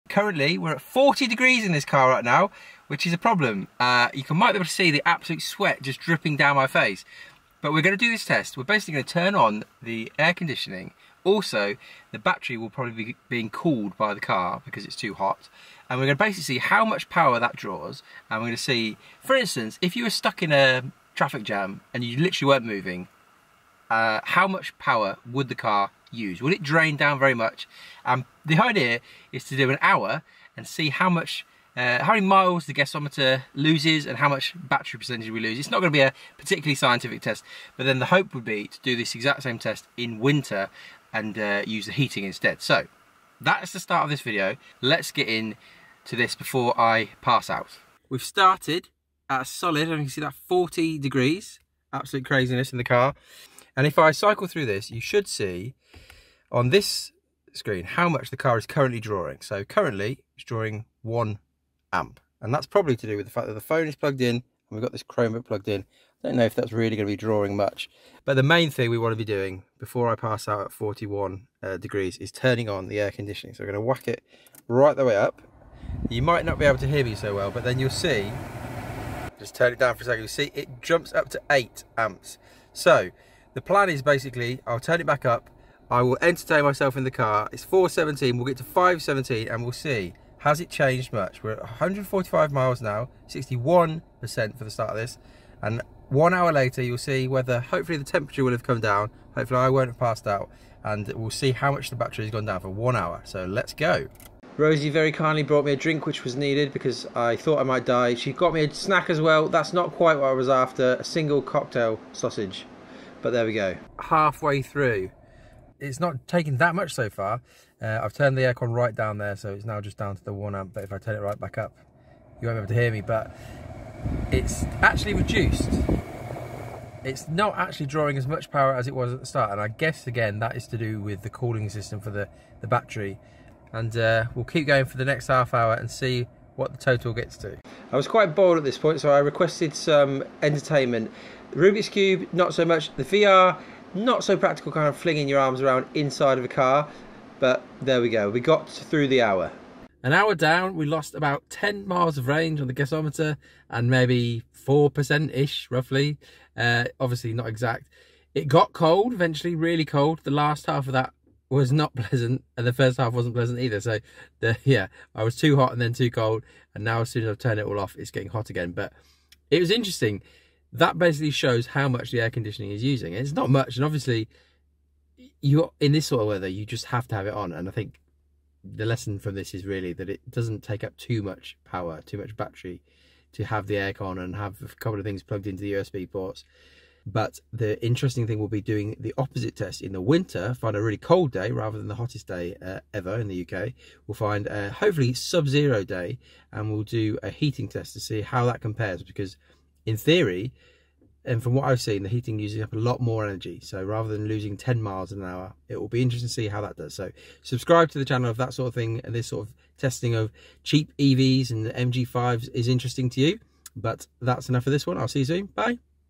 How Much EV Battery Does Air Con Use?
How much battery does air conditioning use in an electric car? In this video I test my MG5 Long Range EV to see how much battery does the air con use up in one hour? I was the hottest day ever recorded in the UK so I decided to spend it in my boiling hot car! It was a very interesting test and I'm looking forward to doing the same test in winter with the heating on!